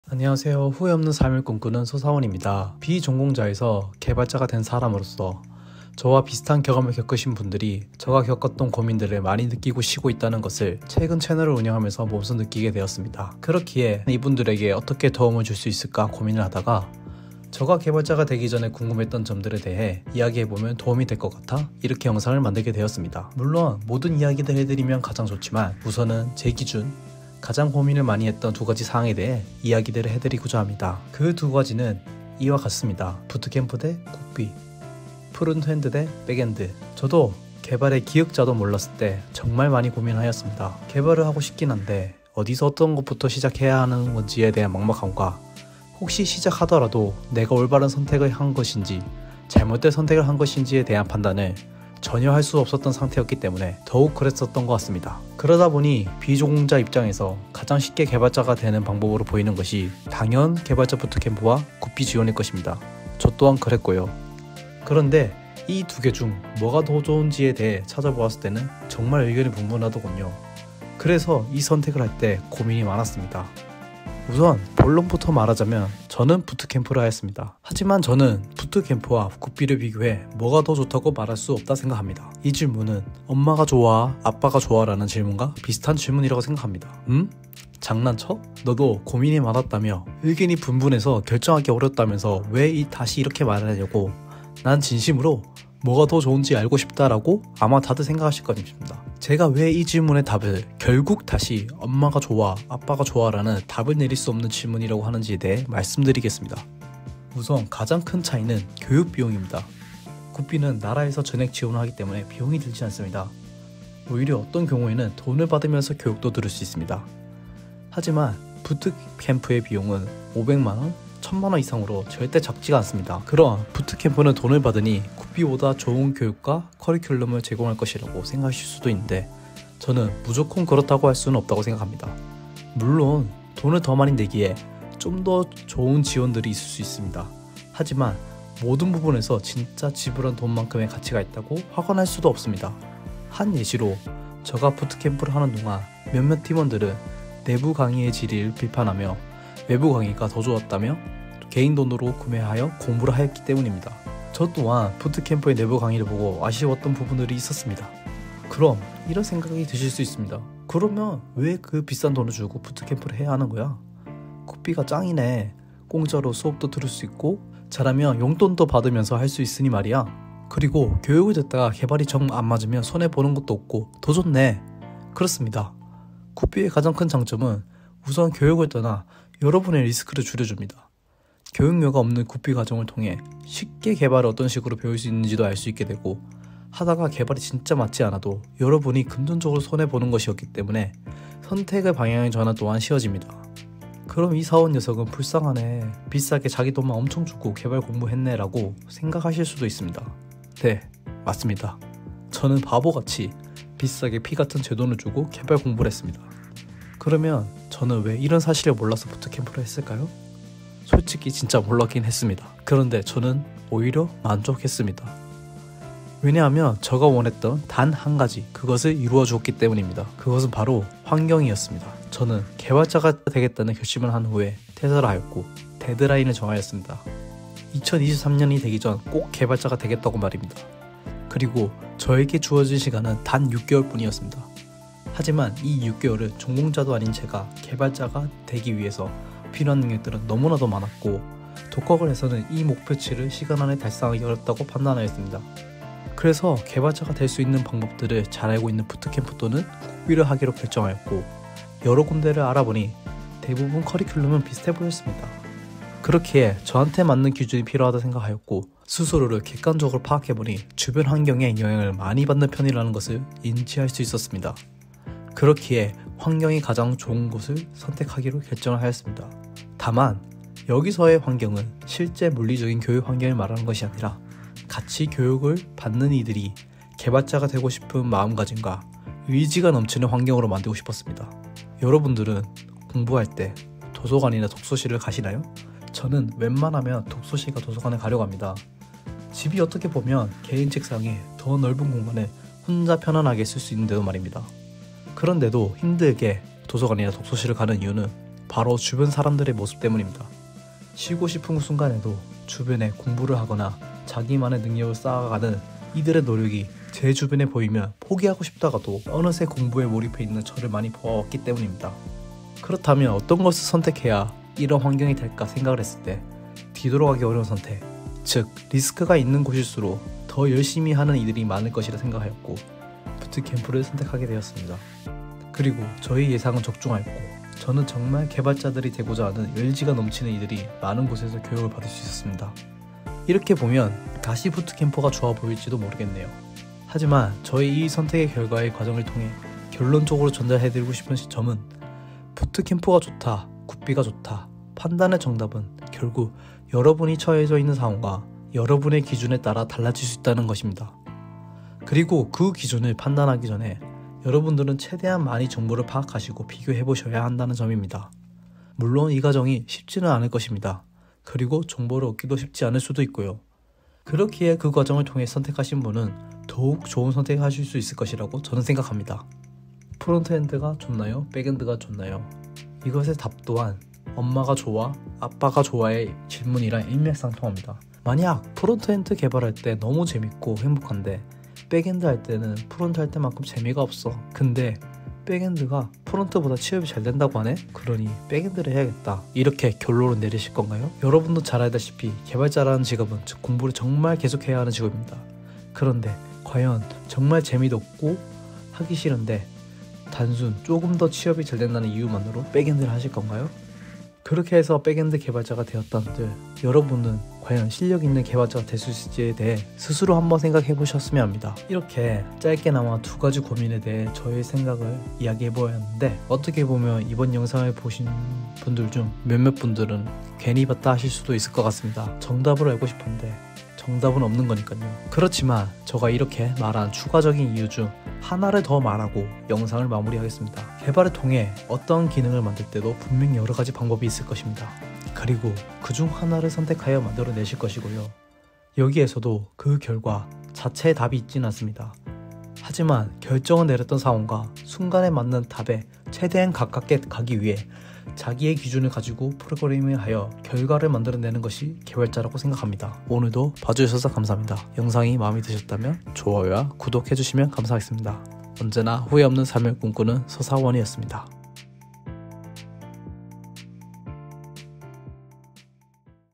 비전공자 출신 개발자 가장 많이 받은 질문 BEST2
비전공자 출신 개발자가 된후 정말 여러가지 질문들을 받게 되었습니다.
그러다보니 저와같으신 분들이 정말 많더라고요
그런분들에게 도움이 되고자 영상을 만들어보았습니다.